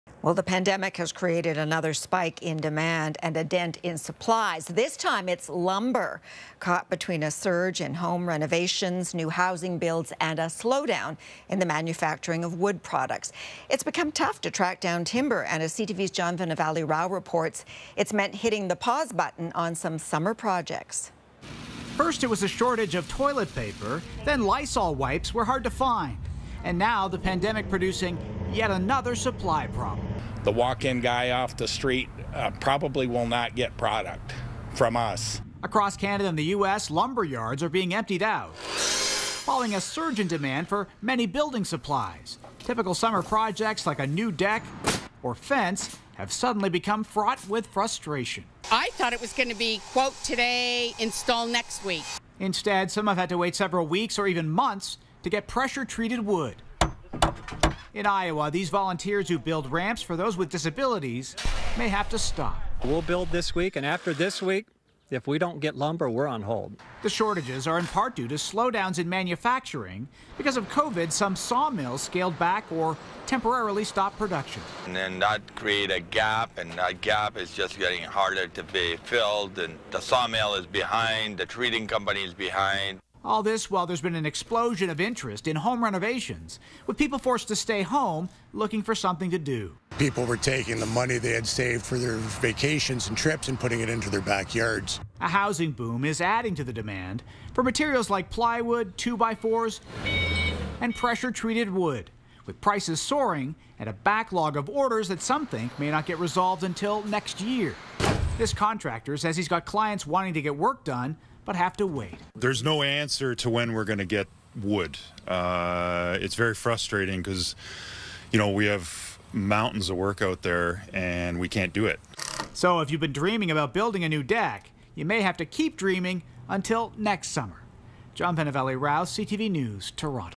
Rising demand for lumber the latest pandemic shortage
As home renovations and new housing surge amid the COVID-19 pandemic, lumber has seen a rise in demand. John VennaVally-Rao explains.